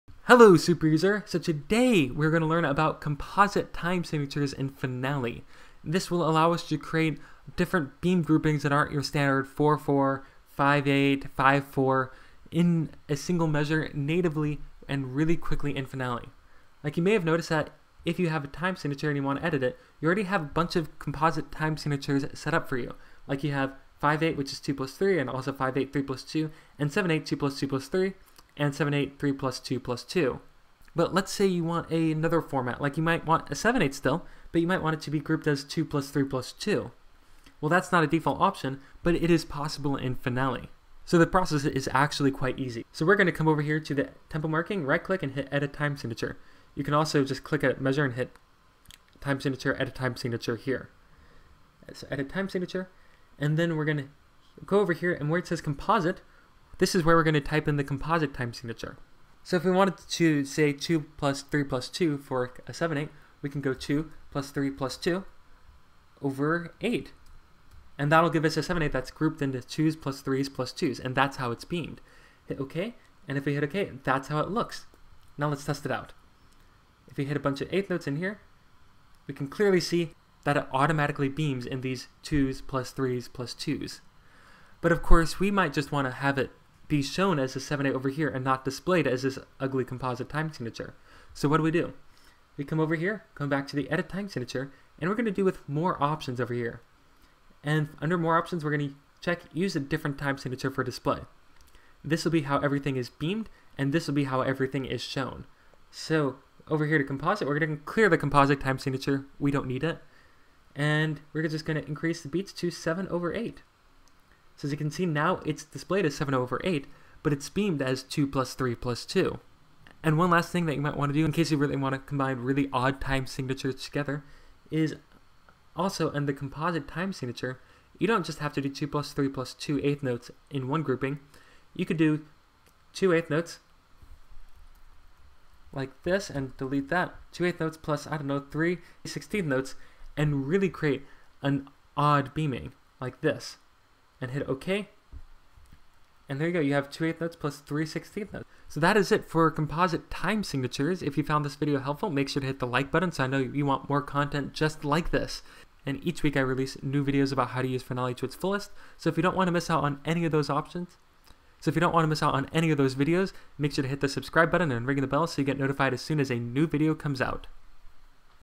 How to Create Composite Time Signatures | Finale Superuser
This is how to create composite time signatures in Finale .